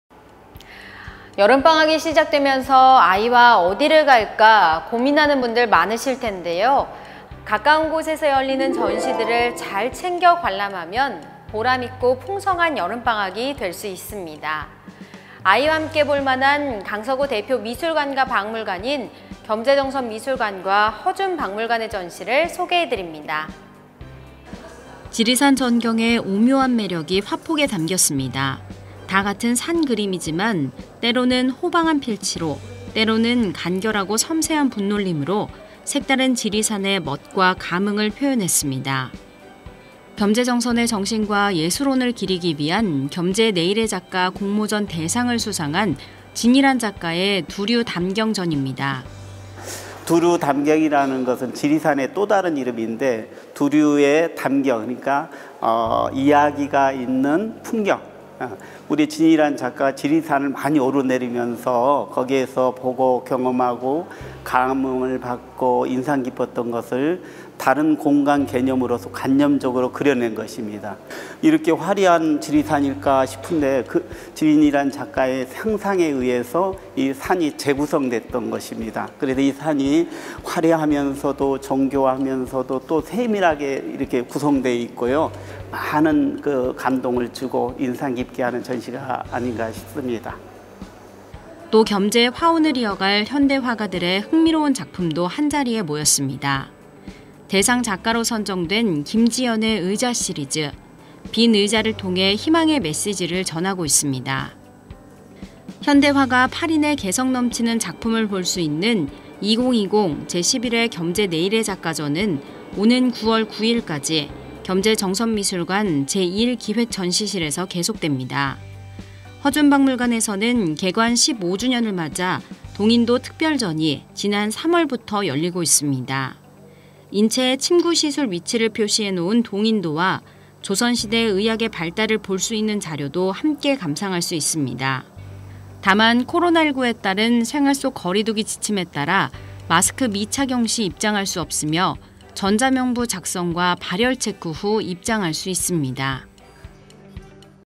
강서구의 대표 미술관, 박물관에서 여름방학 즐겨요
여름방학이 시작되면서 아이와 어디를 갈까 고민하시는 분들 많으실 텐데요.
가까운 곳에서 열리는 전시들을 잘 챙겨 관람하면 보람 있고 풍성한 여름방학이 될 수 있습니다.
아이와 함께 볼만한 강서구 대표 미술관과 박물관인 겸재정선미술관과 허준박물관의 전시회를 소개해드립니다.

지리산 전경의 오묘한 매력이 화폭에 담겨있습니다. 다 같은 산 그림이지만 때로는 호방한 필치로 때로는 간결하고 섬세한 붓놀림으로 색다른 지리산의 멋과 감흥을 표현했습니다.
겸재정선의 정신과 예술혼을 기리기 위한 ‘겸재 내일의 작가’ 공모전 대상을 수상한 진희란 작가의 ‘두류담경展’입니다.

인터뷰 / 겸재정선미술관 김용권 관장
‘두류담경’이라는 것은 지리산의 또 다른 이름인데, 두류(지리산)의 담경은 이야기가 있는 풍경.
우리 진희란 작가가 지리산을 많이 오르내리면서 거기에서 보고 경험하고 감흥을 받고 인상 깊었던 것을 다른 공간 개념으로써 관념적으로 그려낸 것입니다. 이렇게 화려한 지리산일까 싶은데, 진희란 작가의 상상에 의해서 산이 재구성됐던 것입니다. 이 산이 화려하면서도 정교하면서도 또 세밀하게 구성돼 있고요. 많은 감동을 주고 인상 깊게 하는 전시라고 생각합니다.

또 겸재의 화혼을 이어갈 현대 화가들의 흥미로운 작품도 한자리에 모였습니다. 대상 작가로 선정된 김지연의 의자시리즈. 빈 의자를 통해 희망의 메시지를 전하고 있습니다.
현대 화가 8인의 개성 넘치는 작품을 볼 수 있는 ‘2020 제 11회 겸재 내일의 작가 展’은 오는 9월 9일까지 겸재정선미술관 제1기획 전시실에서 계속됩니다.

허준박물관에서는 개관 15주년을 맞아 동인도 특별전이 지난 3월부터 열리고 있습니다. 인체에 침구 시술 위치를 표시해놓은  '동인도(銅人圖)'와 조선시대 의약의 발달을 볼 수 있는 자료도 함께 감상할 수 있습니다.

다만 코로나19에 따른 생활 속 거리두기 지침에 따라 마스크 미착용 시 입장할 수 없으며 전자 명부 작성과 발열체크 후 입장할 수 있습니다.